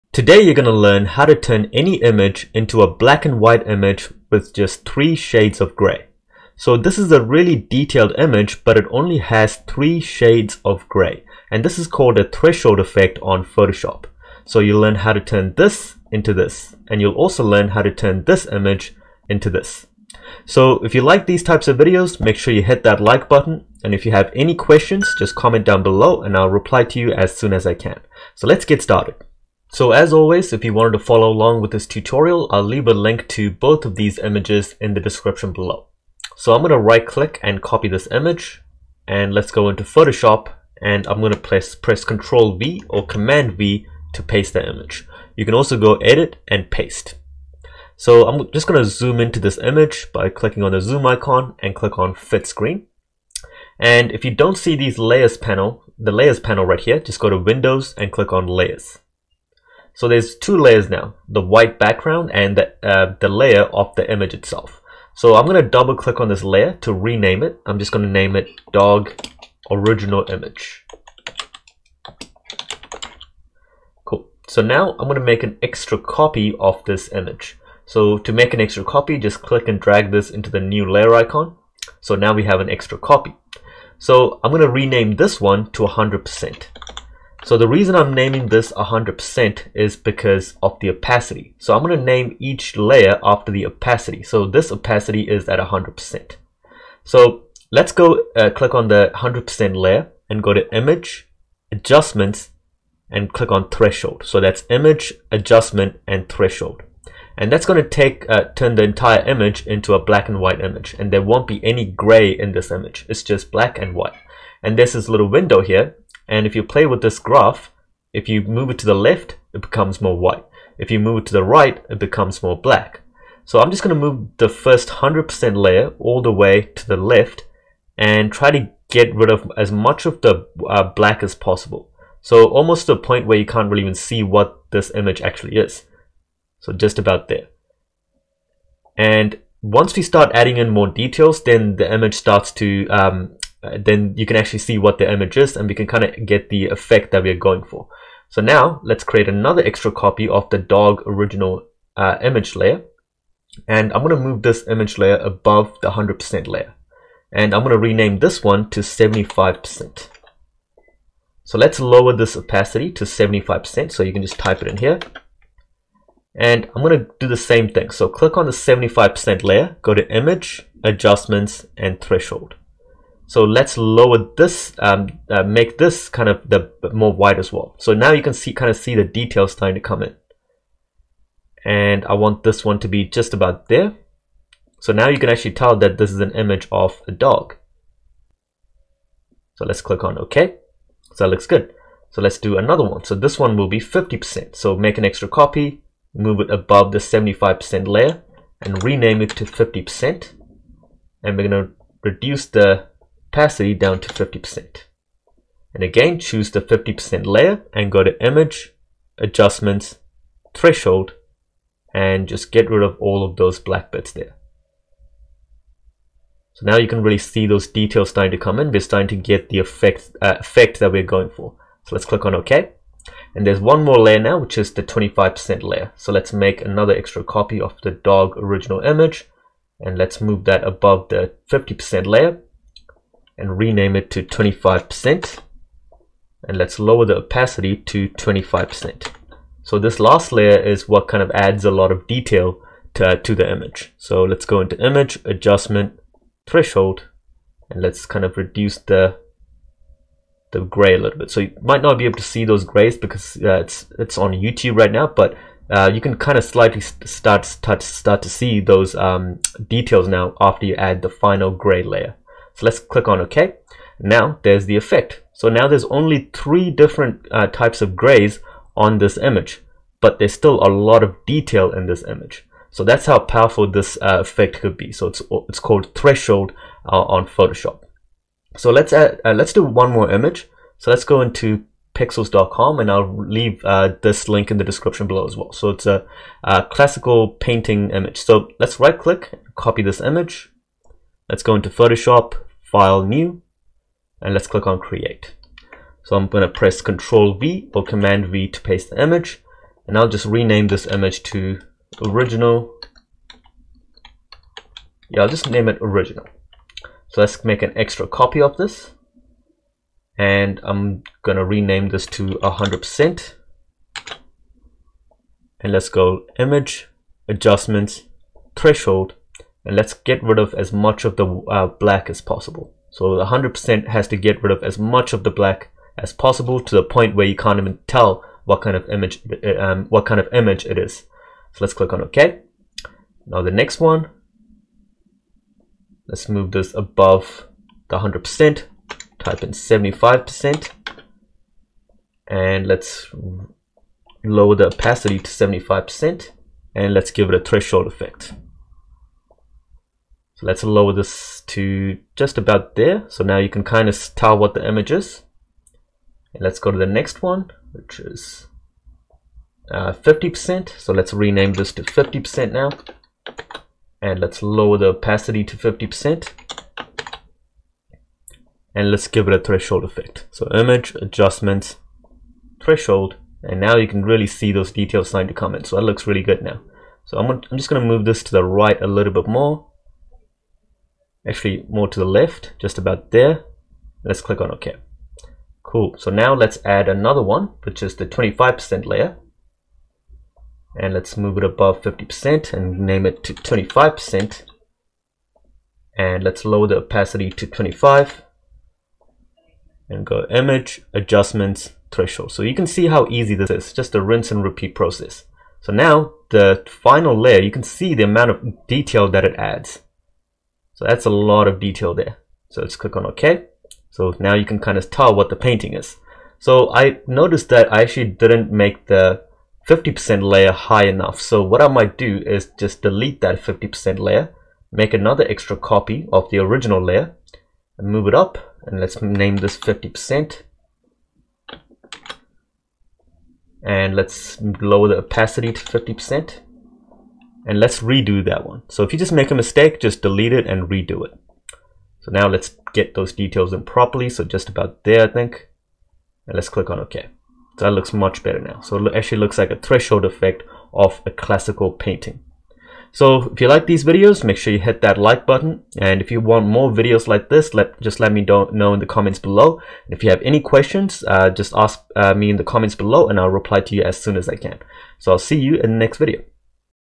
Photoshop Threshold Effect Tutorial | Photoshop Basics For Beginners
In today's video you'll learn how to the threshold effect in Photoshop. This creates a black and white image with only three shades of grey. First you'll how to create a dog portrait and you'll learn how to edit a classical painting.